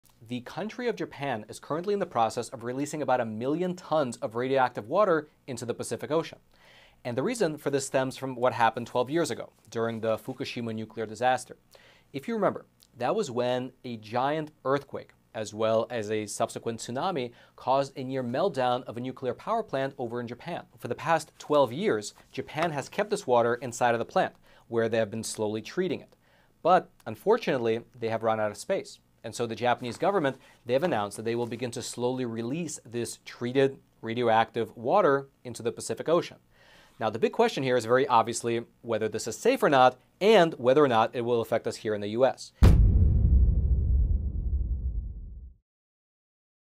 1.3M Tons of Nuclear Wastewater Being Dumped Into Ocean | Shorts | Facts Matter
🔴 EpochTV 25¢ Sale Ends soon

Unbeknownst to most people, Japan is currently in the process of releasing about a million tons of radioactive water into the Pacific Ocean.

The reason for this stems from what happened 12 years ago, during the Fukushima nuclear disaster. An earthquake and a subsequent tsunami caused a near meltdown of a nuclear power plant in Japan. The situation was a complete mess: the reactors shut down, and the backup generators that fuelled the cooling system lost power, which then led to three nuclear meltdowns and the contamination of a million tons of water.

For the past 12 years, Japan has kept this water inside of the plant where they have been slowly treating it, but they have now run out of space. And so, the Japanese government has announced that they will begin to slowly release this treated radioactive water into the Pacific Ocean.

Now the big questions here are: is this safe? And how will it affect us here in the United States?

1.3M Tons of Nuclear Wastewater Being Dumped Into Ocean | Shorts | Facts Matter